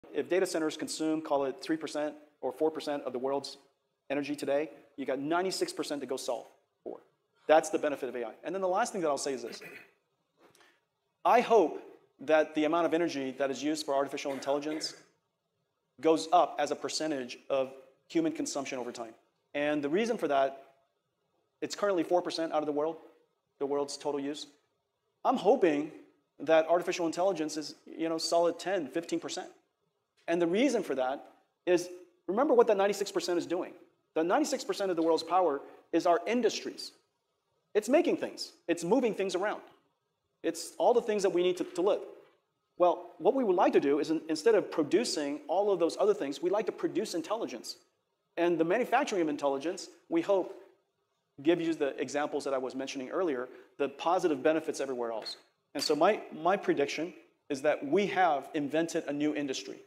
🔋 AI’s Impact on Energy & Industry | Jensen Huang on the Future of AI & Power Consumption ⚡🌍
💡 AI & Energy: A New Industrial Revolution? 🚀

Jensen Huang (NVIDIA CEO) & Jim Kavanaugh (WWT CEO) discuss how AI will reshape global energy consumption and revolutionize industries. 🌍⚡

🔹 Key Takeaways from This Discussion
✅ AI & Data Centers: AI currently consumes 3-4% of global energy, but this share is expected to increase to 10-15% as AI becomes a core industry.
🔋 AI as an Energy Investment: Instead of consuming energy for traditional industries (manufacturing, logistics, etc.), AI enables the production of intelligence.
🏭 Industrial Transformation: AI will optimize manufacturing, supply chains, and logistics, making industries more efficient and sustainable.
🌎 AI & Green Energy: Advanced AI will drive clean energy solutions, optimizing power grids, renewables, and energy storage.
🔮 A New Industry Emerges: AI is not just a tool but an entirely new industry, shaping the future of automation, productivity, and sustainability.

🔹 How AI’s Energy Shift Affects the World
🌟 More Efficient Factories: AI-driven automation will cut waste, reduce emissions, and optimize industrial production.
⚡ Smarter Power Grids: AI will manage energy demand, predict usage, and reduce energy waste, making global power systems more sustainable.
🚀 AI as the Next Big Industry: Just like the internet revolutionized commerce, AI will become a central pillar of modern economies.

💬 Do you think AI will become a core industry, surpassing traditional sectors?